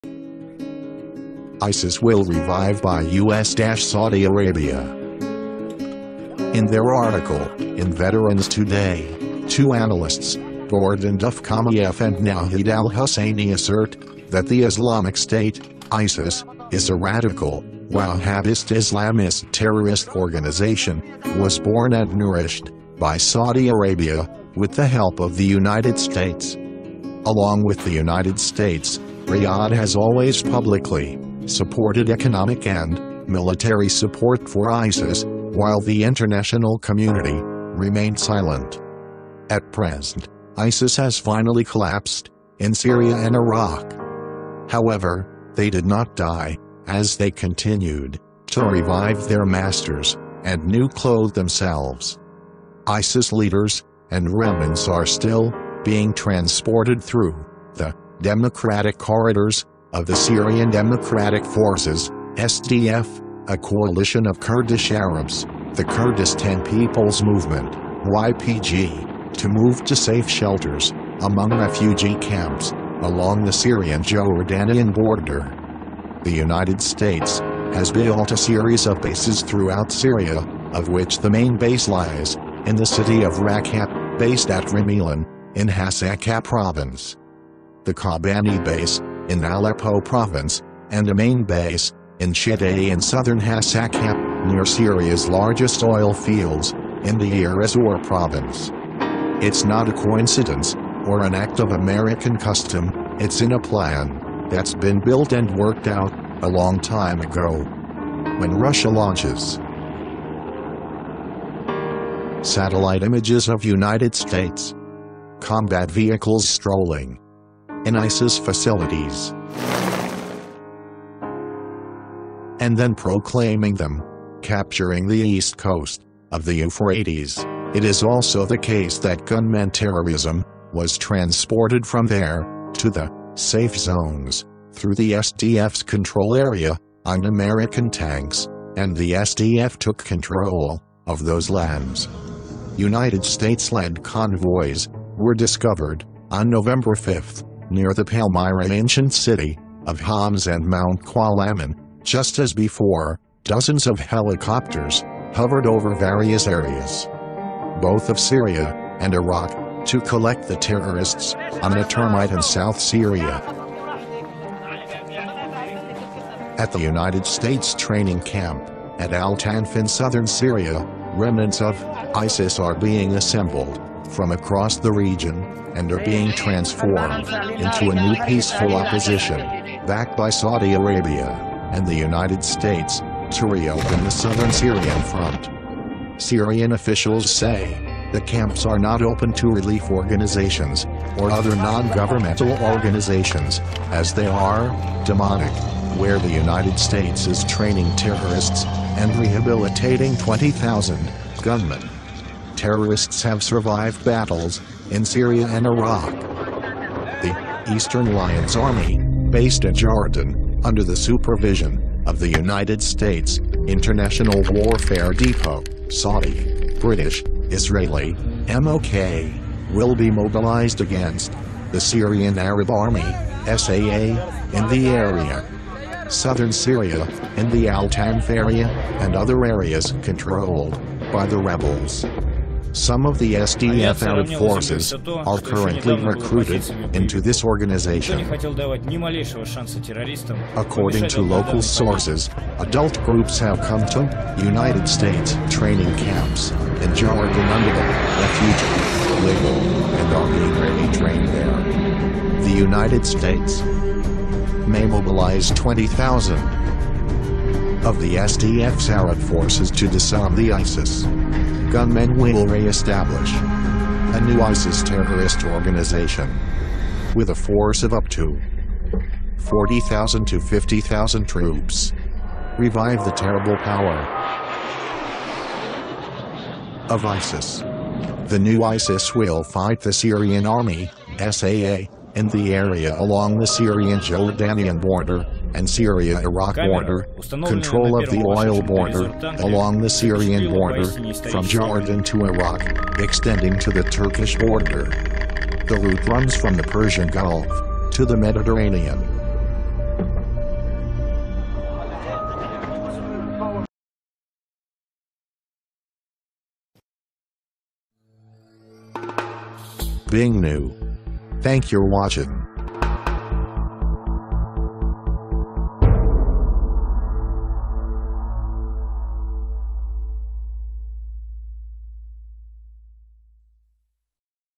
Plan B : Replace the new coat for ISIS
ISIS will revive by US-Saudi Arabia.

In their article, in Veterans Today, two analysts, Gordon Duf,f and Nahed al Husaini assert, that the Islamic State, (ISIS) is a radical, Wahhabist Islamist terrorist organization, was born and nourished, by Saudi Arabia, with the help of the United States.

Along with the United States, Riyadh has always publicly, supported economic and, military support for ISIS, while the international community, remained silent.

At present, ISIS has finally collapsed, in Syria and Iraq.
However, they did not die, as they continued, to revive their masters, and new clothe themselves.

ISIS leaders, and remnants are still, being transported through, the "Democratic Corridors", of the Syrian Democratic Forces, (SDF), a coalition of Kurdish Arabs, the Kurdistan People's Movement, (YPG), to move to safe shelters, among refugee camps, along the Syrian Jordanian border.

The United States, has built a series of bases throughout Syria, of which the main base lies, in the city of Raqqa; Based at Rimelan, in Hasakah province. the Kobani base, in Aleppo province, and a main base, in Sheddai in southern Hasakah, near Syria's largest oilfields, in Deir Ezzor province.

It's not a coincidence, or an act of American custom, it's in a plan, that's been built and worked out, a long time ago.

When Russia launches, satellite images of US, combat vehicles strolling, in ISIS facilities, and then proclaiming them, capturing the East Coast, of the Euphrates, it is also the case that gunmen Terrorism, was transported from there, to the "safe zones", through the SDF's control area, on American tanks, and the SDF took control, of those lands.

US led convoys, were discovered, on November 5, near the Palmyra ancient city, of Homs and Mount Qualamon, just as before, dozens of helicopters, hovered over various areas. both of Syria, and Iraq, to collect the terrorists, on a termite in South Syria.
At the US training camp, at al Tanf in southern Syria, remnants of, ISIS are being assembled, from across the region, and are being transformed, into a new peaceful opposition, backed by Saudi Arabia, and the United States, to reopen the southern Syrian front.

Syrian officials say, the camps are not open to relief organizations, or other non-governmental organizations, as they are "demonic" - where the US is training terrorists, and rehabilitating 20,000, gunmen. Terrorists have survived battles, in Syria and Iraq.

The "Eastern Lions Army", based in Jordan, under the supervision, of the United States, International Warfare Depot, Saudi, British, Israeli (M O K), will be mobilized against, the Syrian Arab Army  (SAA), in the area. southern Syria, in the al Tanf area, and other areas controlled, by the rebels.

Some of the SDF Arab forces, are currently recruited, into this organization. According to local sources, adult groups have come to, US training camps, in Jordan under the, "refugee" label, and are being re-trained there.

The United States, may mobilize 20 thousand, of the SDF's Arab forces to disarm the ISIS. Gunmen will re establish, a new ISIS terrorist organization, with a force of up to, 40,000 to 50,000 troops; revive the terrible power, of ISIS.

The new ISIS will fight the Syrian Army, (SAA), in the area along the Syrian Jordanian border, and Syria Iraq border, control of the oil border, along the Syrian border, from Jordan to Iraq, extending to the Turkish border. the route runs from the Persian Gulf, to the Mediterranean).